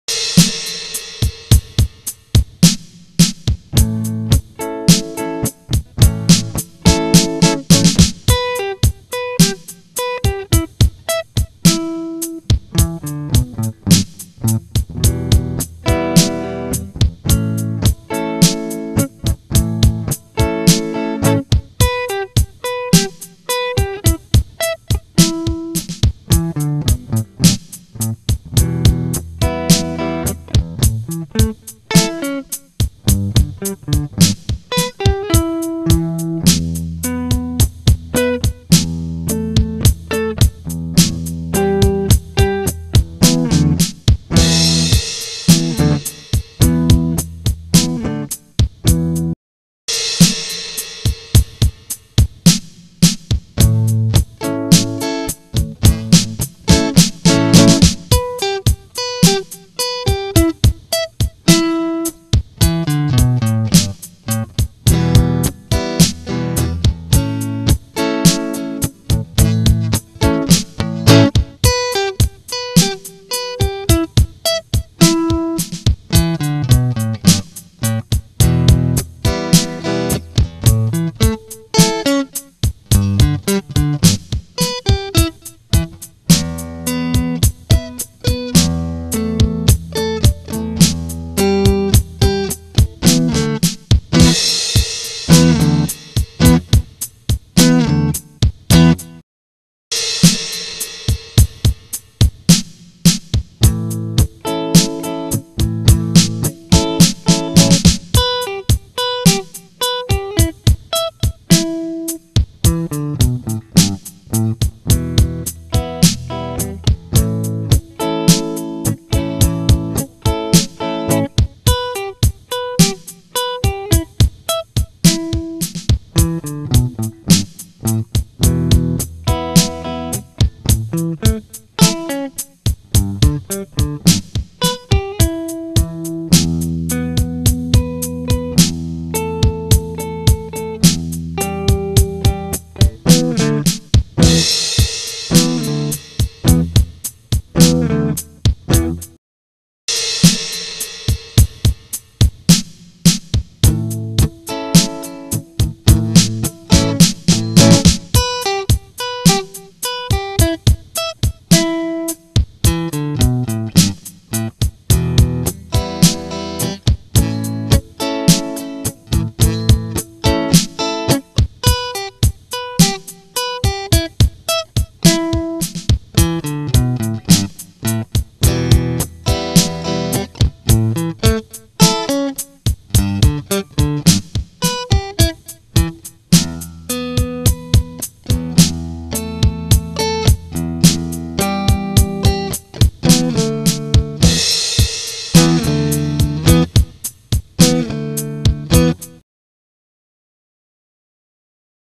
bandstop175 Zguitars Roland Tradition (neck and middle) (A .... Em .... )
Roland G-808 Neck position guitar
Tradition stratocaster Neck position guitar
Roland G-808 Middle position guitar
Tradition stratocaster Middle position guitar
1band song 6 ... beat 53
(no microphone)